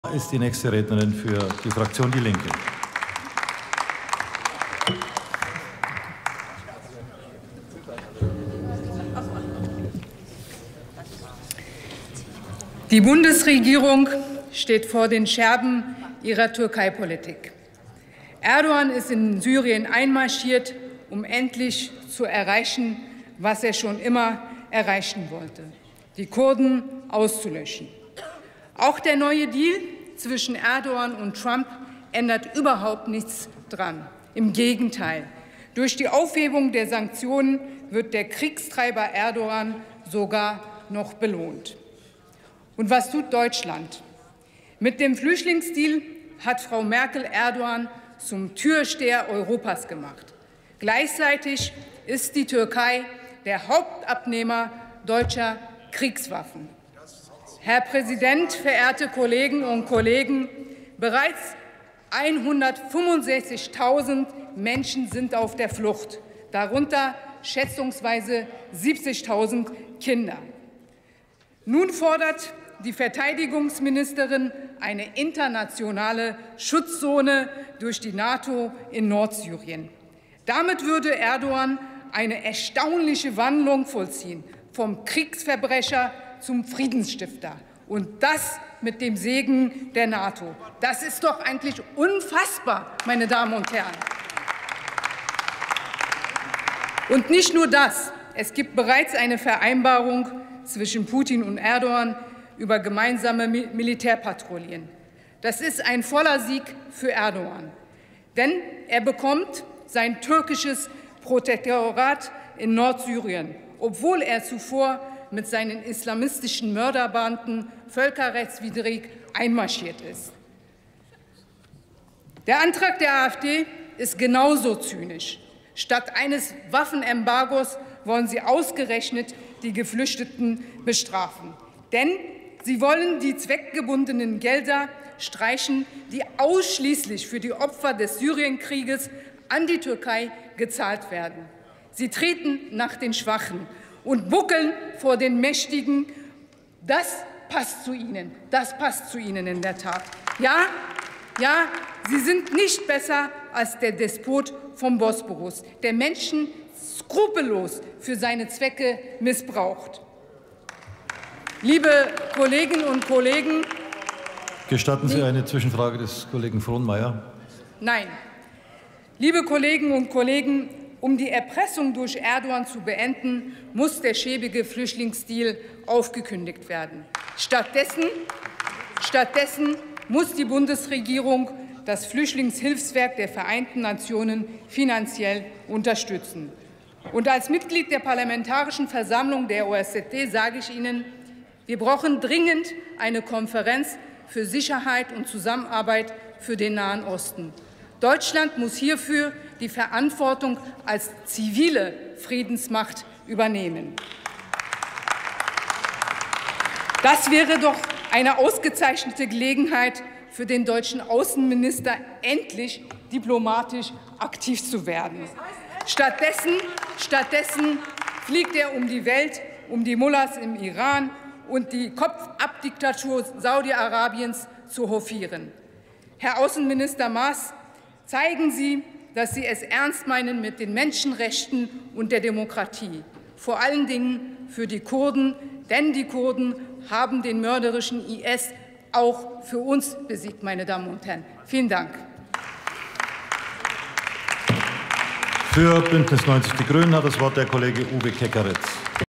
Helin Evrim Sommer // DIE LINKE:NATO- Sicherheitszone in Nordsyrien wäre ein Sieg für Erdogan.
"Die AfD tritt nach den Schwachen und buckelt vor den Mächtigen. Der Flüchtlingsdeal mit der Türkei muss aufgekündigt werden. Deutschland muss das Flüchtlingshilfswerk der Vereinten Nationen stärker unterstützen. Eine NATO- Sicherheitszone in Nordsyrien hilft den Menschen nicht, sondern wäre ein voller Sieg für Erdogan.

Wir brauchen dringend eine Konferenz für Sicherheit und Zusammenarbeit für den Nahen Osten. Deutschland muss hier die Verantwortung als zivile Friedensmacht übernehmen!"